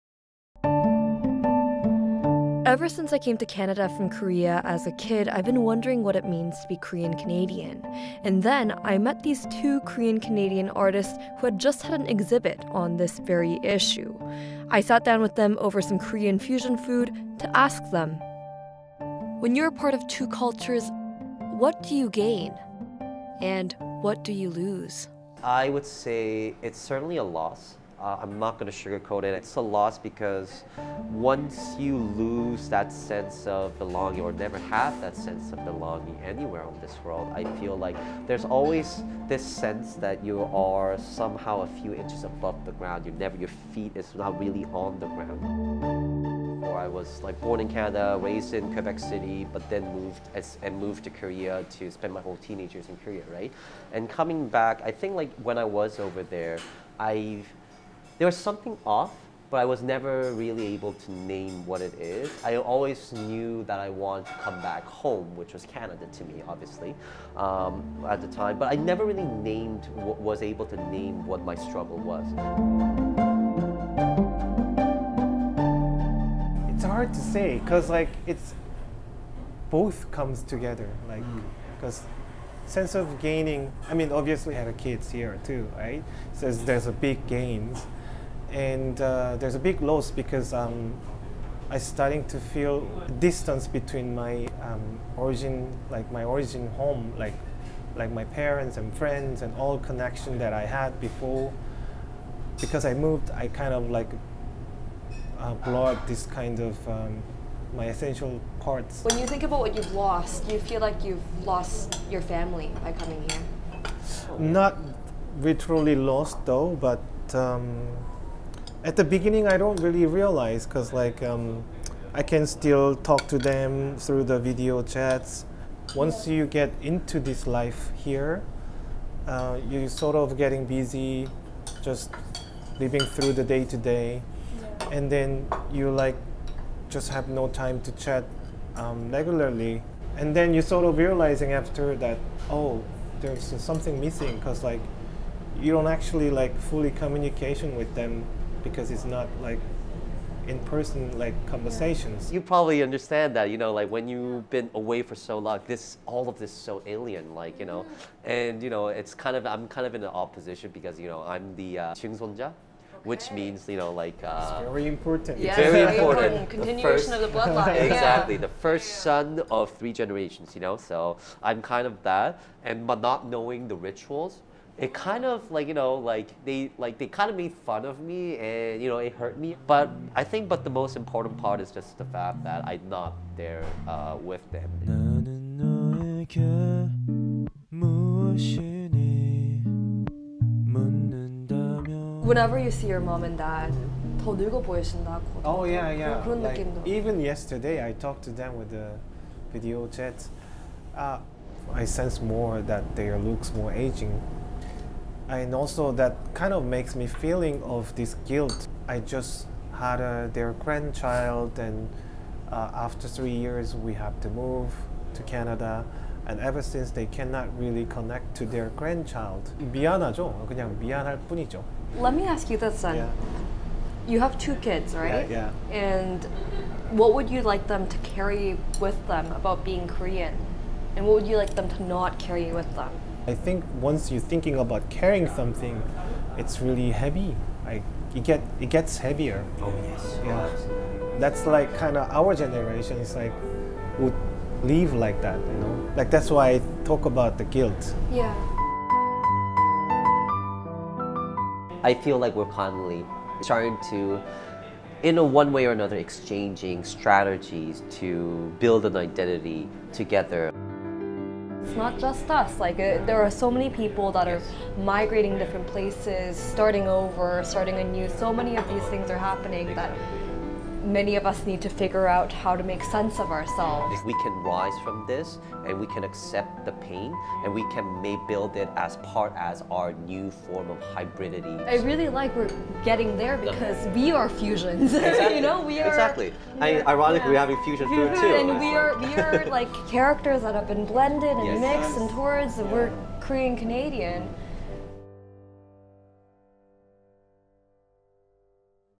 What is gained and lost when immigrating to Canada? | Carrying Our Cultures
Immigration brought blessings for Kevin Park Jung-Hoo and Jin Hee Woong, but it also came at a cost. Reporter Jennifer Yoon sits down with the two Korean Canadian artists to discuss these feelings over a meal of Korean fusion food.

Carrying Our Cultures comes from a desire to uplift Asian stories in Quebec and provide an intimate look into the Asian Canadian experience.